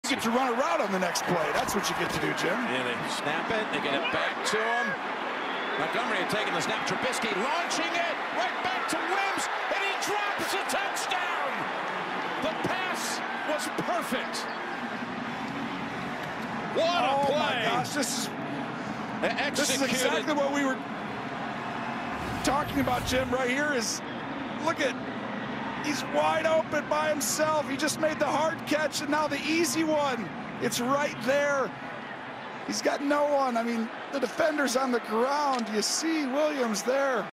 Mitchell Trubisky PERFECT Pass & Wims Drops WIDE Open Touchdown | Bears vs Saints
NFL Playoffs | Wild Card Round